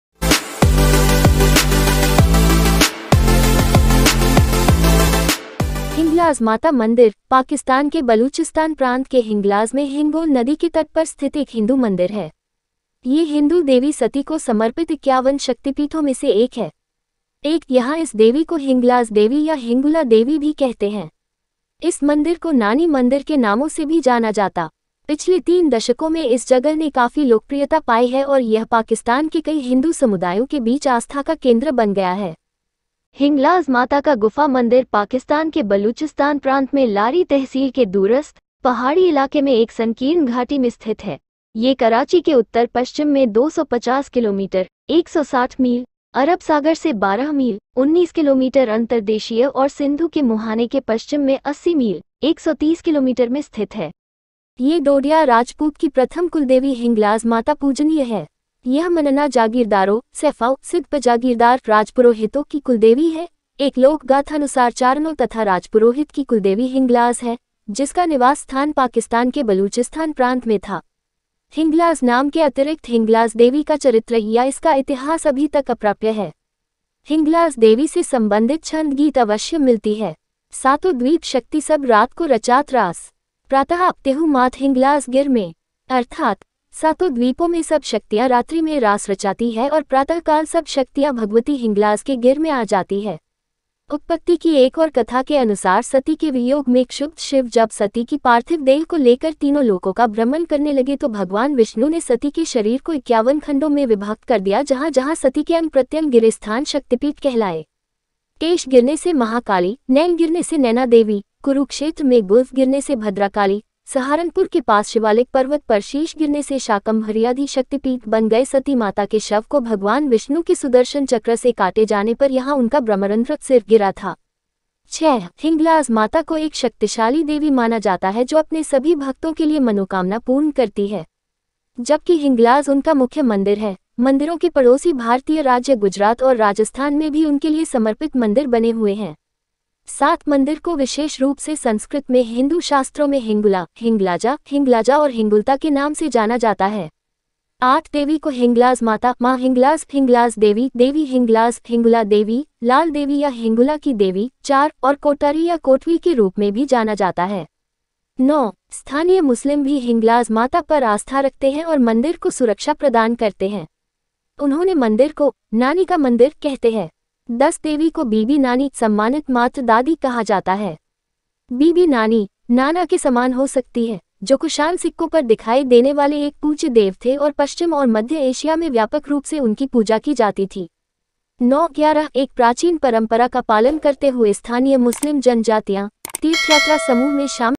hinglaj devi temple in pakistan | hinglishstream, hinglajmaa | nani ka mandir Hindu madir Pakistan
| hinglaj devi temple in pakistan | hinglishstream,| hinglajmaa  | nani ka mandir हिंगलाज माता मन्दिर, पाकिस्तान के बलूचिस्तान प्रान्त के हिंगलाज में हिंगोल नदी के तट पर स्थित एक हिन्दू मन्दिर है। यह हिन्दू देवी सती को समर्पित इक्यावन शक्तिपीठों में से एक है।[1] यहाँ इस देवी को हिंगलाज देवी या हिंगुला देवी भी कहते हैं। इस मन्दिर को नानी मन्दिर के नामों से भी जाना जाता । पिछले तीन दशकों में इस जगह ने काफी लोकप्रियता पाई है और यह पाकिस्तान के कई हिंदू समुदायों के बीच आस्था का केन्द्र बन गया है।[2]हिंगलाज माता का गुफा मंदिर पाकिस्तान के बलूचिस्तान प्रांत में लारी तहसील के दूरस्थ, पहाड़ी इलाके में एक संकीर्ण घाटी में स्थित है। यह कराची के उत्तर-पश्चिम में 250 किलोमीटर (160 मील), अरब सागर से 12 मील (19 किमी) अंतर्देशीय और सिंधु के मुंहाने के पश्चिम में 80 मील (130 किमी) में स्थित है। यह हिंगोल नदी के पश्चिमी तट पर, मकरान रेगिस्तान के खेरथार पहाड़ियों की एक शृंखला के अंन्त में बना हुआ है।[3][4] यह क्षेत्र हिंगोल राष्ट्रीय उद्यान के अन्तर्गत आता है।[5]